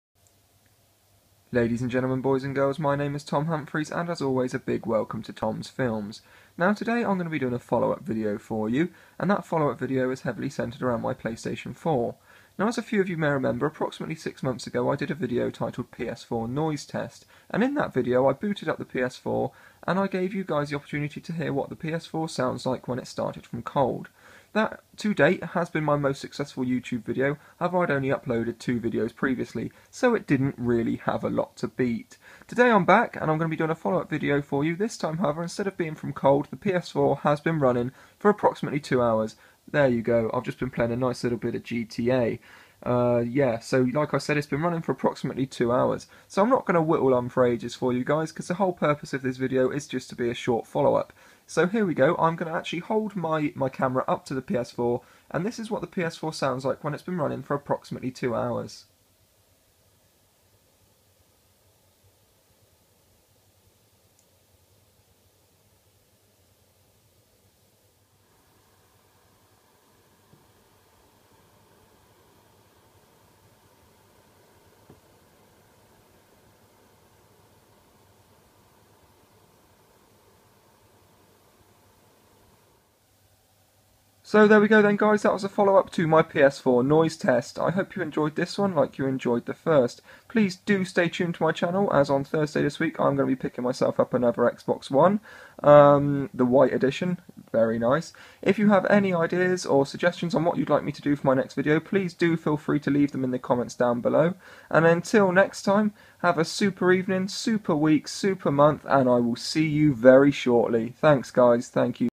PS4 noise test 2 (UK)
A follow up to my previous PS4 noise test.  However this time the PS4 had been running for around two hours.